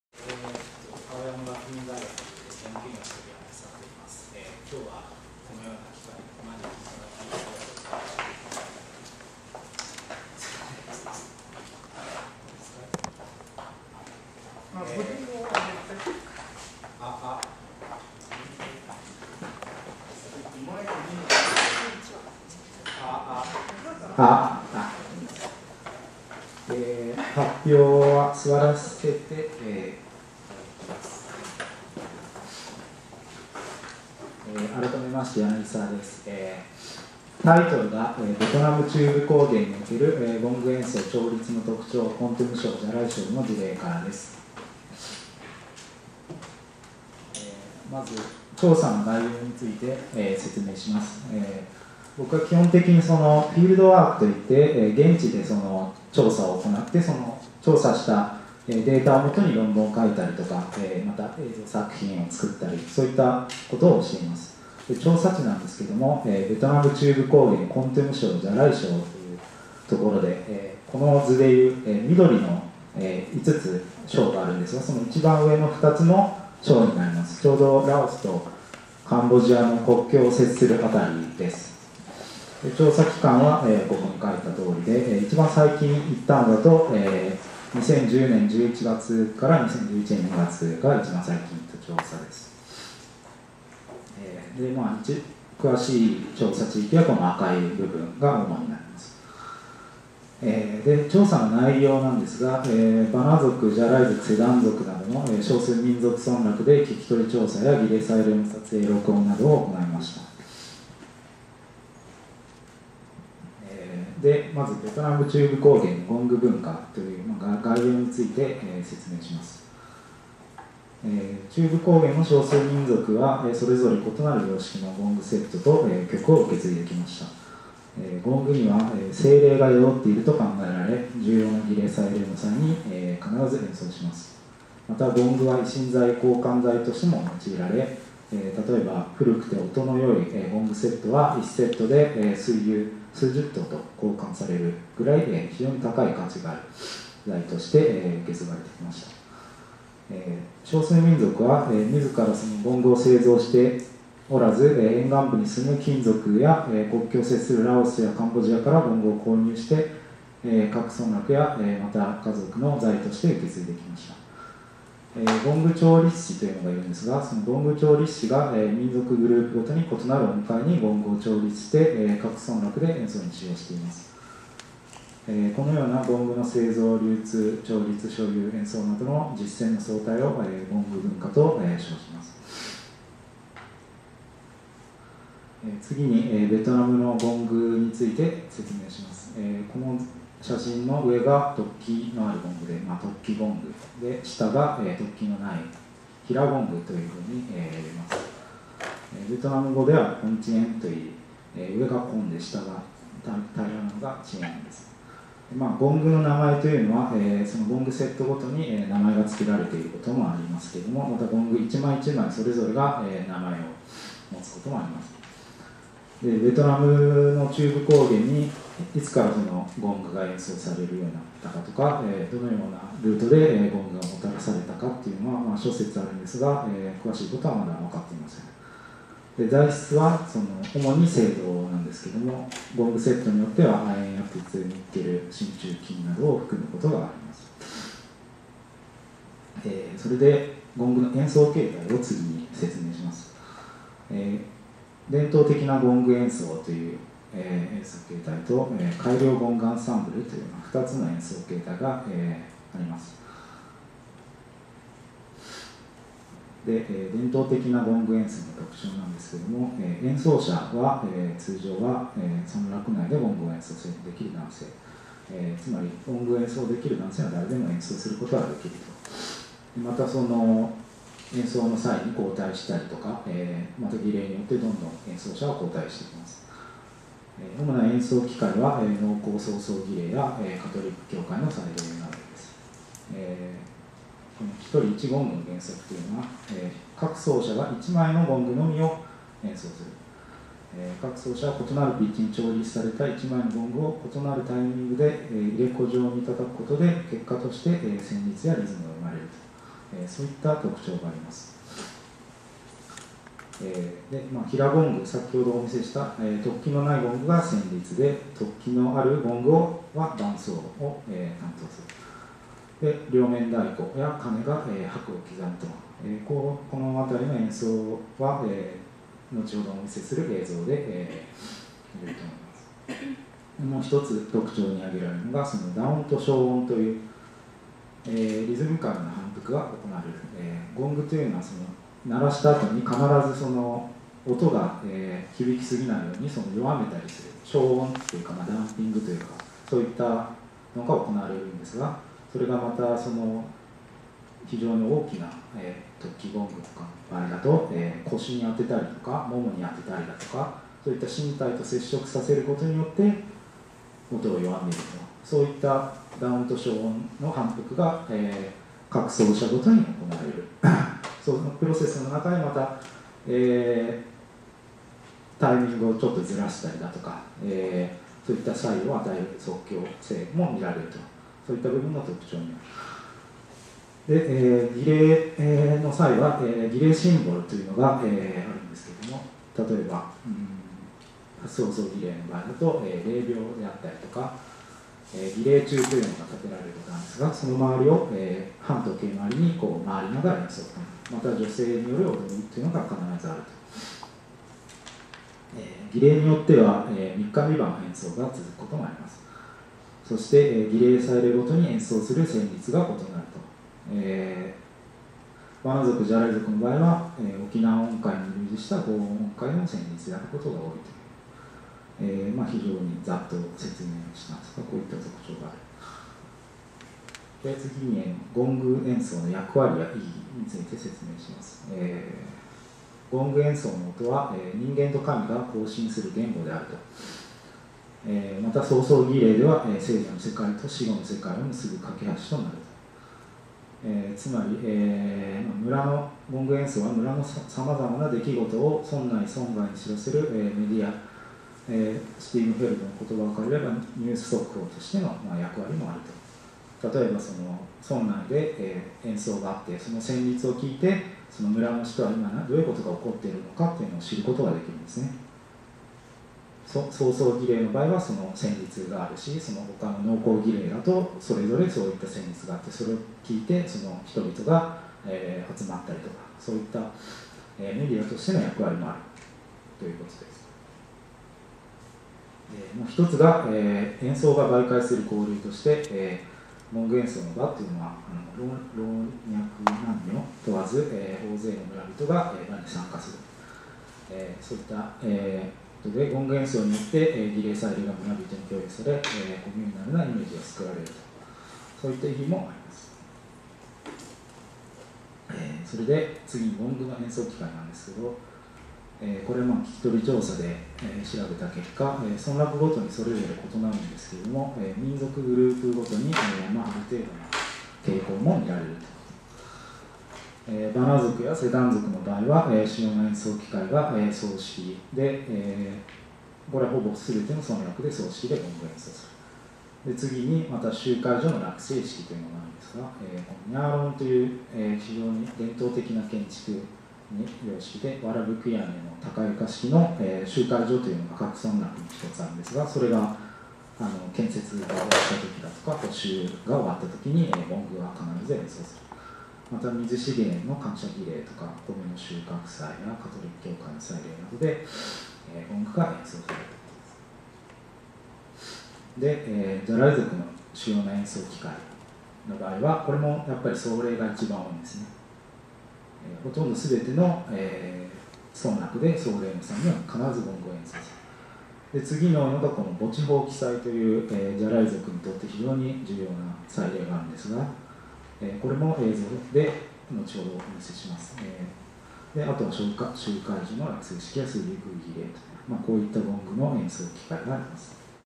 JASMIM2011 lecture YANAGISAWA Eisuke#1
2011第3回日本音楽即興学会大会、「ベトナム中部高原のゴング文化」：柳沢英輔
The Japanese Association for the Study of Musical IMprovisation